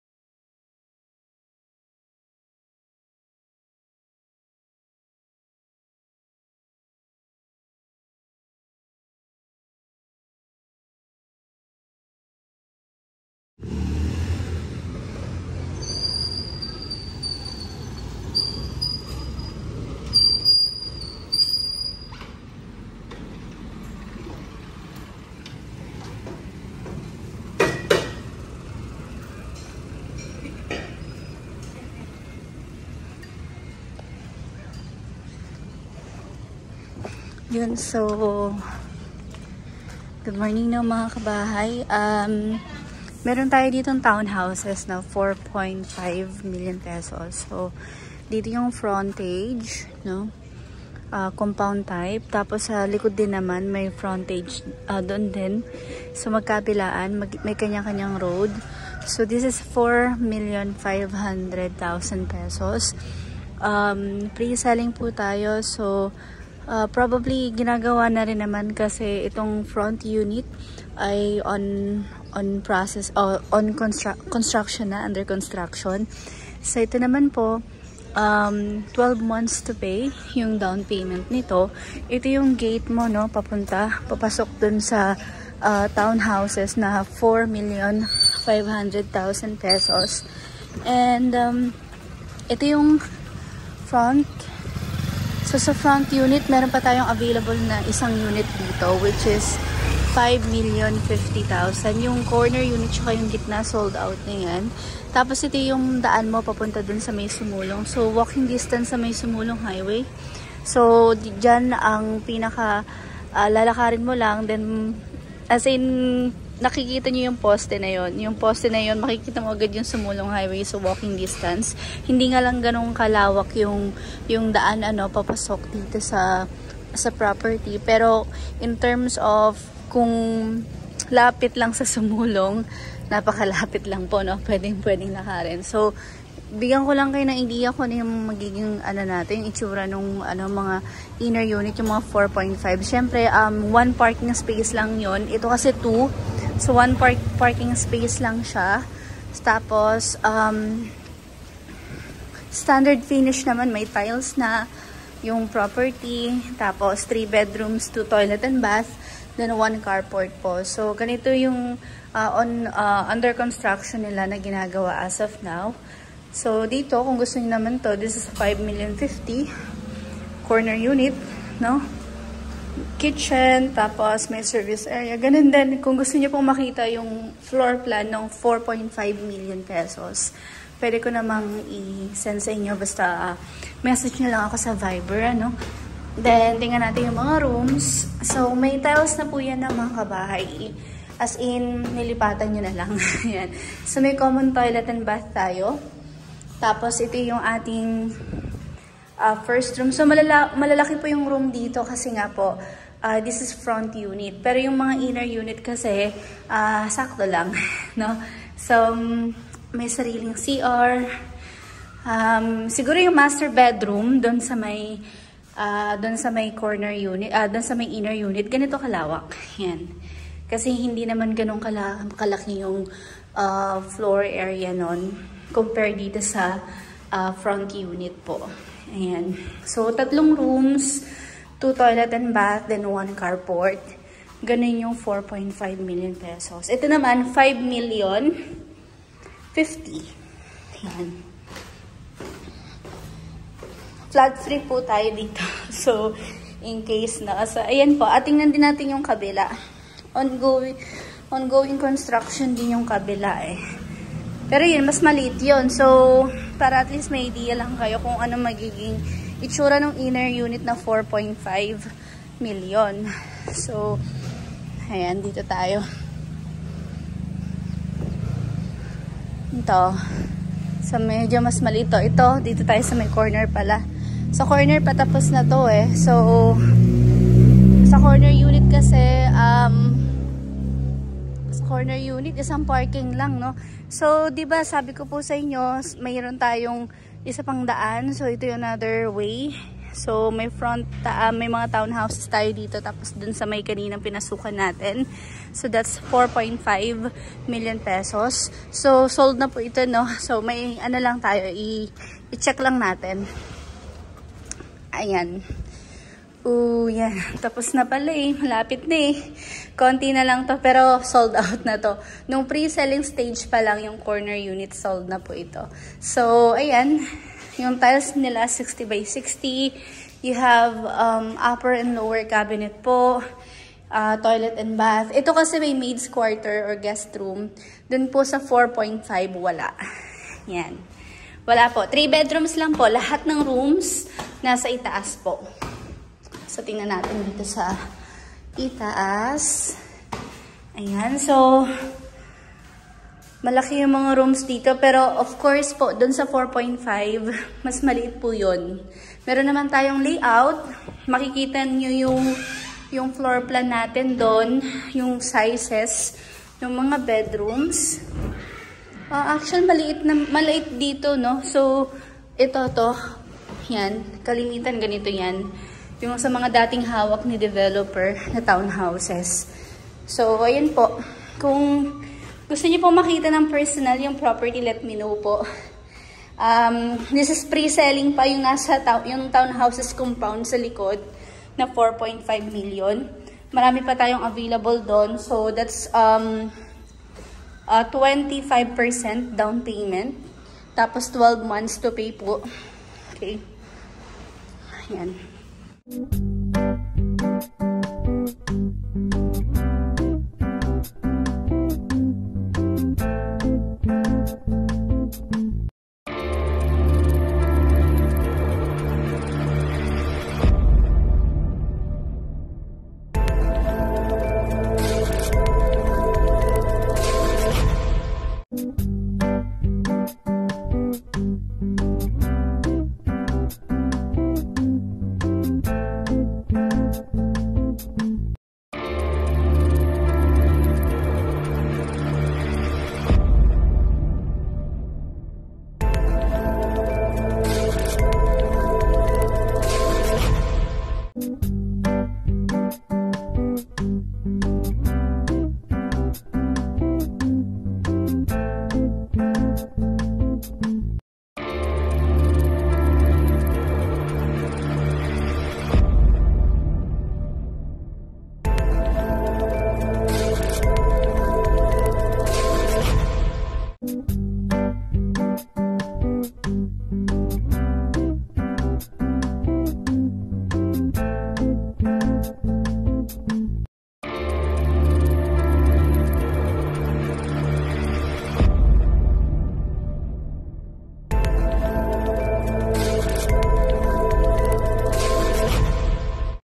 P4.5M | Affordable Townhomes for Sale in Sumulong hi-way Mambugan Antipolo City near SM Masinag
P4.5M | Affordable Townhomes for Sale in Sumulong hi-way Mambugan Antipolo City

 Absolutely flood safe!
 Near LRT & SM Masinag
 Near Meralco and Major Commercial establishments.
 Just 1 ride to Marikina Public Market,Antipolo City Hall
Walking distant to Jeepney line transpo Cubao ,Marikina

Lot area: 51 sqm to 52 sqm
Floor area:70sqm
- 3 Bedrooms,2 toilet & bath, 1 Carport
- 3 Bedrooms, 2 toilet & bath, 2 Carports

Price packages:
Corner 1 -SOLD
Front Middle - SOLD
Corner Side - SOLD

ONLY AVAIL INNER UNIT

4,500,000
4,550,000
4,650,000

Terms:
50k Reservation
25% Down payment - 8 months to pay
75% Balance thru Bank Financing